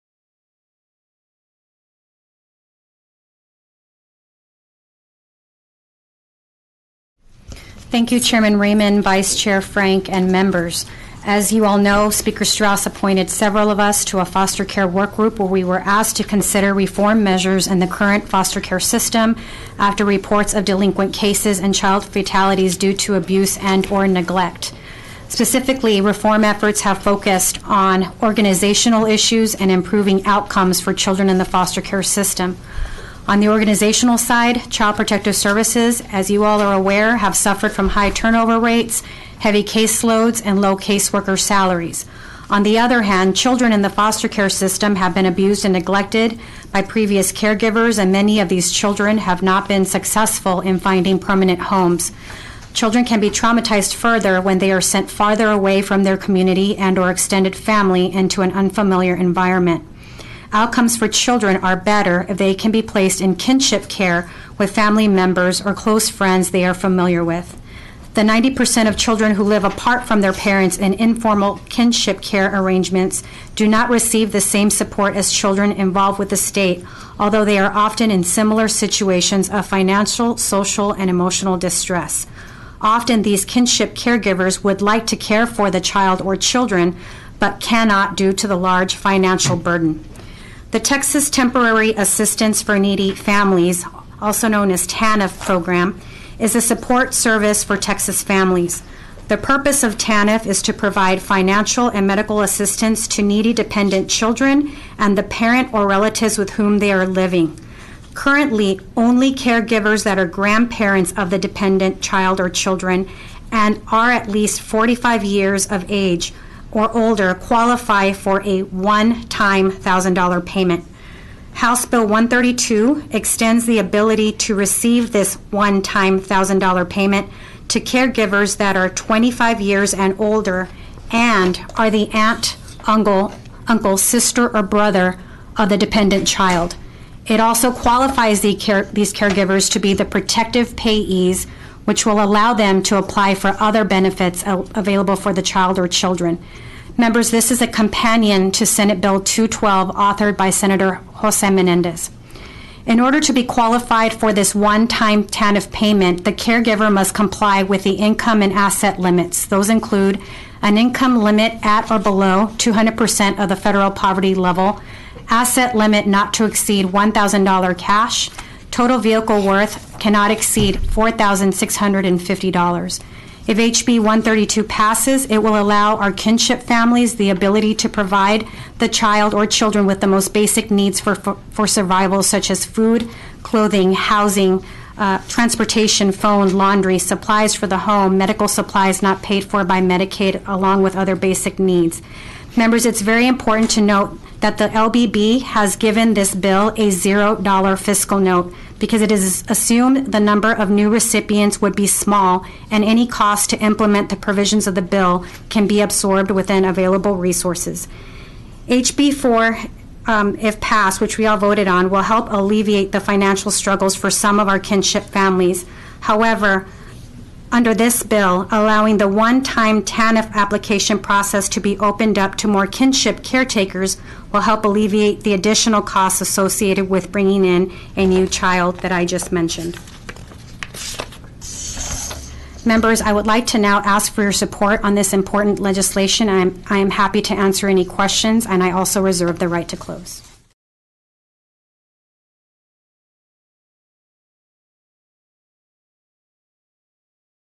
House Human Services - Rep. Minjarez Lays Out HB 132 - March 6, 2017
Rep. Ina Minjarez (HD-124) lays out HB 132, "relating to the ability of certain relative caretakers of dependent children to receive supplemental financial assistance and be assigned as protective payees for financial assistance payments," before the House Committee on Human Services, March 6, 2017.